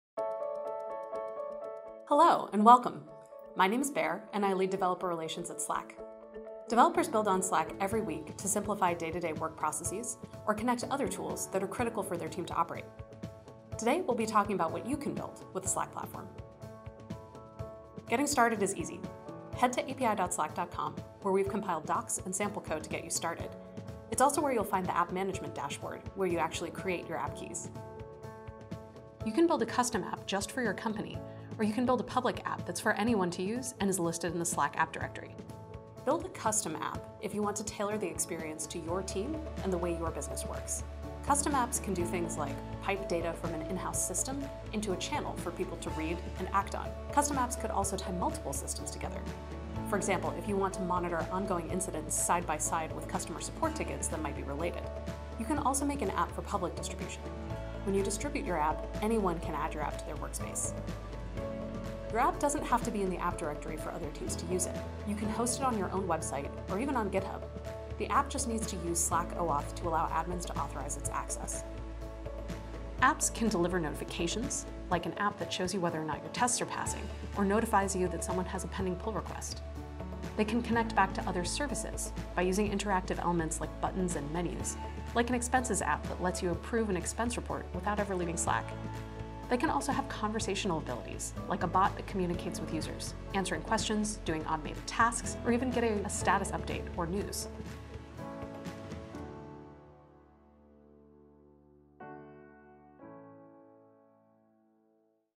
How to get started | Building apps on Slack
Did you know you can build apps on Slack? Make work simpler for your team by connecting critical information and processes to Slack. Create engaging app experiences that enable quick, contextual action on tasks from all of your work tools. Getting started is simple at api.slack.com.